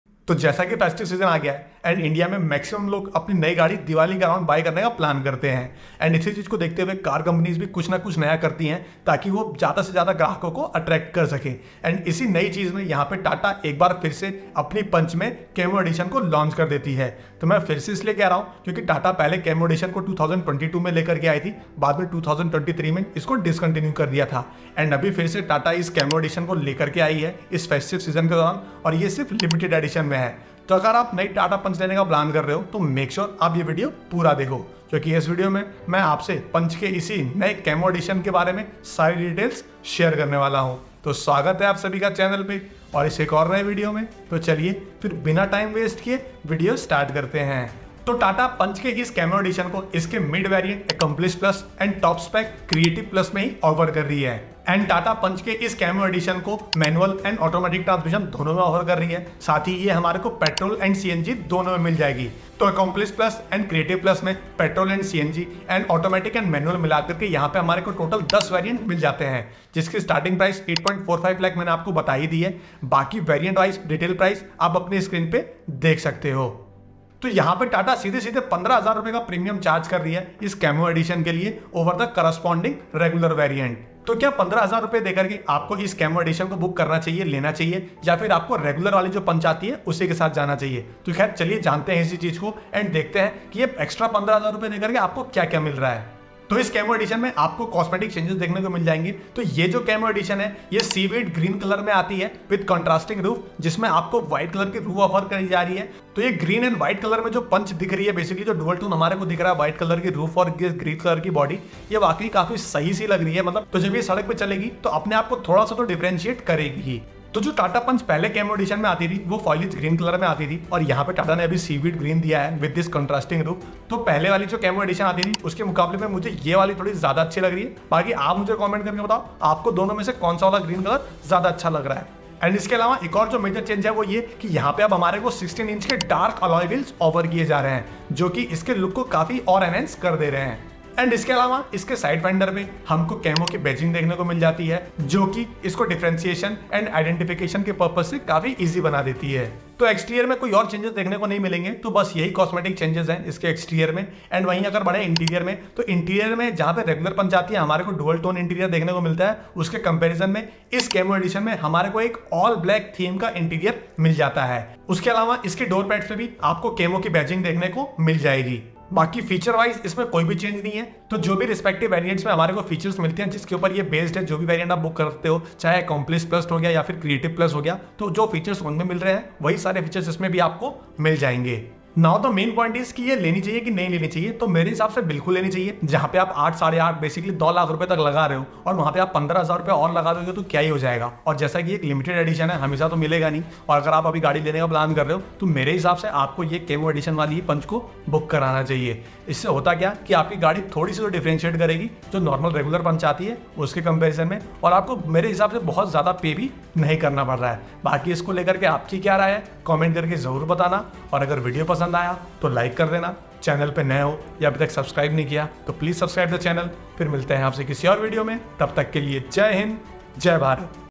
Tata Punch Camo Edition 2024: Limited Edition SUV at ₹8.45 Lakh – New Features & Price Breakdown!
Welcome to our in-depth review of the Tata Punch Camo Edition 2024 – the highly anticipated limited edition SUV from Tata Motors, priced starting at ₹8.45 lakh! In this video, we dive into all the exciting new features, design updates, and exclusive details of this special festive season launch.

💥 What’s New in the Tata Punch Camo Edition 2024?
Tata has introduced a fresh new Seaweed Green paint with a dual-tone option featuring a contrasting white roof. This limited edition comes with stylish 16-inch dark-grey alloy wheels and unique ‘Camo’ badges on the front fenders, giving it a rugged, premium look.

Inside, the Camo Edition brings an all-black interior theme, updated seat upholstery with exclusive ‘Camo’ graphics, blacked-out door handles, and enhanced tech features like a 10.25-inch touchscreen infotainment system, a semi-digital driver’s display, auto climate control, and a wireless phone charger.

🔥 Available Variants & Prices

🔋 Engine & Powertrain Options
The Tata Punch Camo Edition is available with both petrol and CNG options:

88hp, 1.2-litre 3-cylinder petrol engine (MT and AMT)
74hp CNG version (only MT)
🏆 Key Features of Tata Punch Camo Edition:

New Seaweed Green paint shade
Camo-themed black interior with unique upholstery
Advanced safety features: Dual airbags, ABS with EBD, rear parking camera & sensors
Petrol & CNG variants with manual and AMT gearboxes
🚘 Tata Punch vs Rivals
The Punch Camo Edition competes with rivals like the Hyundai Exter, Citroen C3, Maruti Fronx, and Toyota Taisor. How does it stack up? Watch to find out!

✨ Special Festive Offer
This limited edition is available in select units during the festive season, making it a must-watch for SUV enthusiasts looking for something exclusive this year!